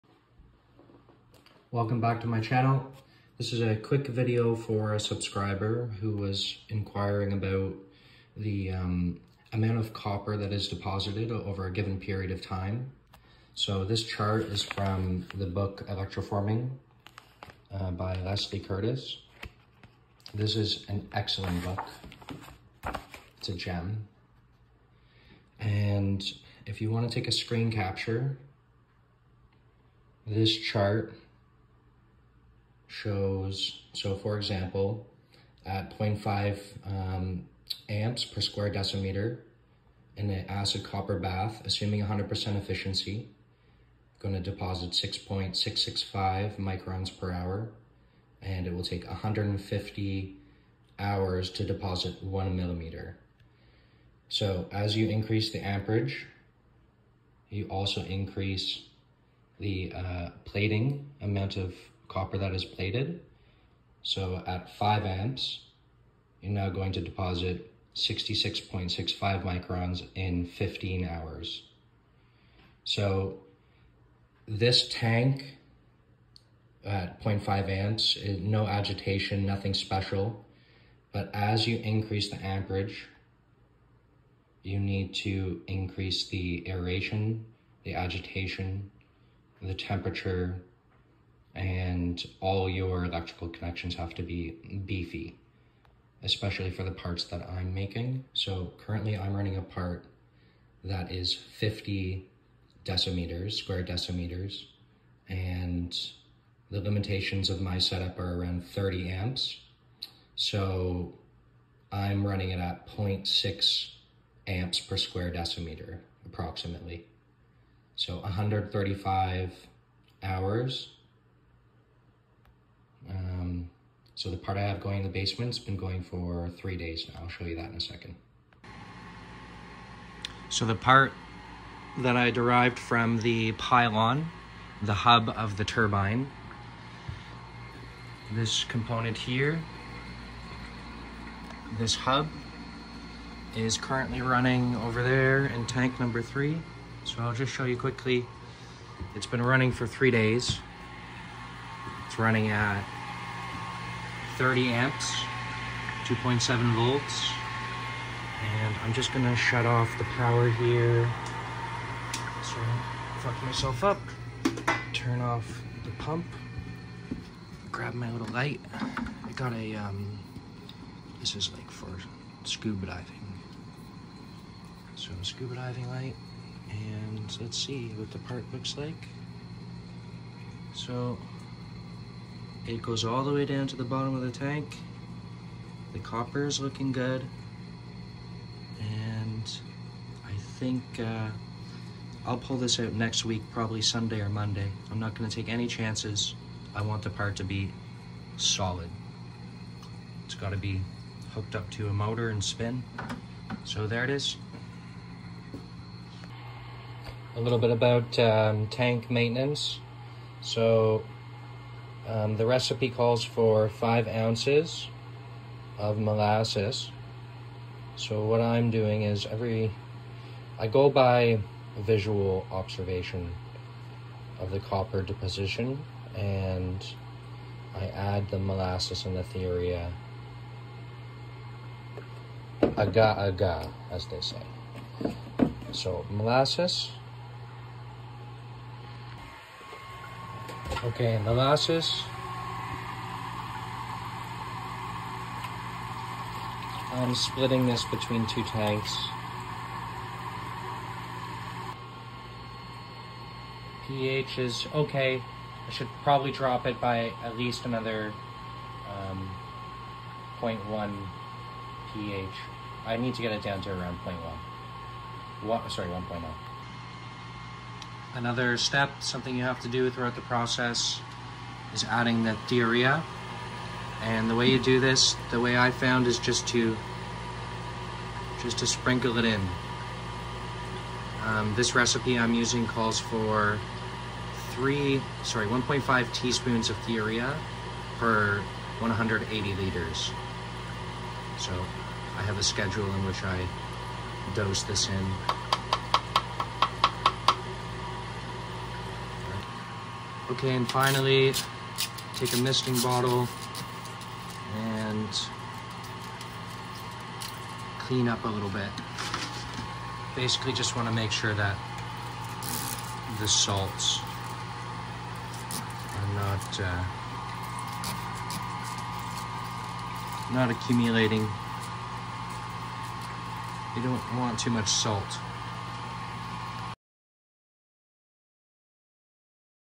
Copper Deposition Rates
Another video here, addressing a question by the subscriber Victor Schauberger regarding how much copper is deposited over a given period of time. Filmed all over the place sorry about that.